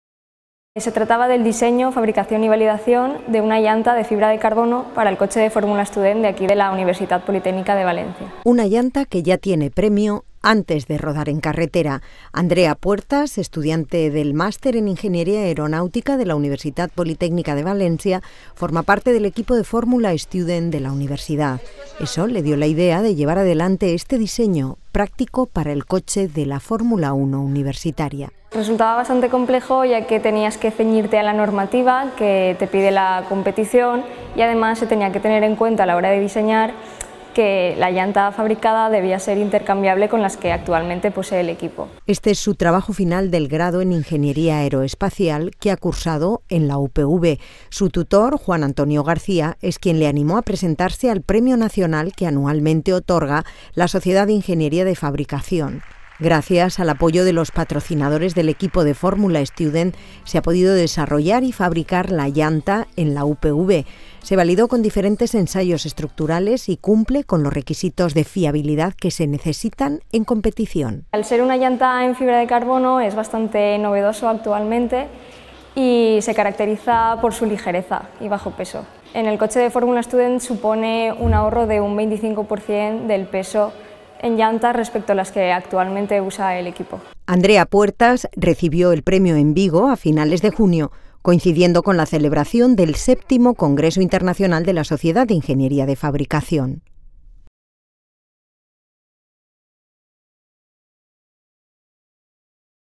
Andrea Puertas, premio nacional TFG Sociedad Ingeniería Fabricación - Noticia @UPVTV 06-07-2017
Una llanta que ya tiene premio antes de rodar en carretera. Andrea Puertas, estudiante del Máster en Ingeniería Aeronáutica de la Universitat Politècnica de València forma parte del equipo de Fórmula Student de la universidad. Eso le dio la idea de llevar adelante este diseño, práctico para el coche de la fórmula uno universitaria.

Este es su trabajo final del grado en Ingeniería Aeroespacial, que ha cursado en la UPV. Su tutor, Juan Antonio García es quien le animó a presentarse al premio nacional que anualmente otorga la Sociedad de Ingeniería de Fabricación. Gracias al apoyo de los patrocinadores del equipo se ha podido desarrollar y fabricar la llanta en la UPV. Se validó con diferentes ensayos estructurales y cumple con los requisitos de fiabilidad que se necesitan en competición.

Andrea Puertas recibió el premio en Vigo, a finales de junio, coincidiendo con la celebración7º Congreso Internacional de la Sociedad de Ingeniería de Fabricación.